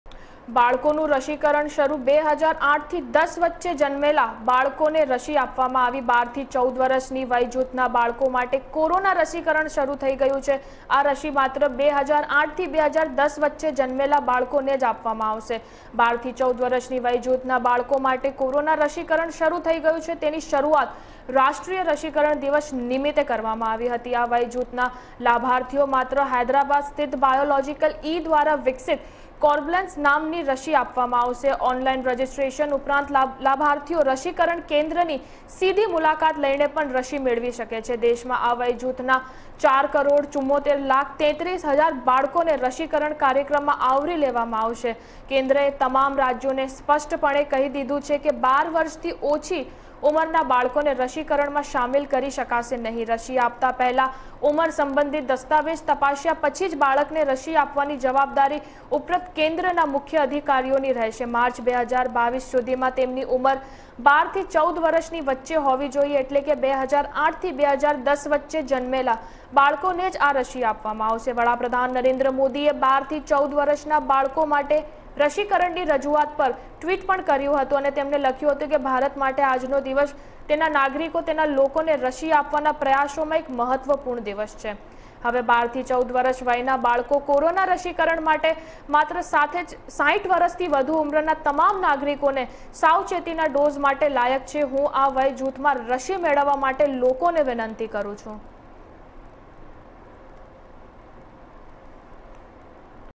બાળકોનું રસીકરણ શરૂ - India News Gujarat
બાળકોનું રસીકરણ શરૂ
2008-10 વચ્ચે જન્મેલાને જ રસી
12 થી 14 વર્ષના બાળકો માટે રસી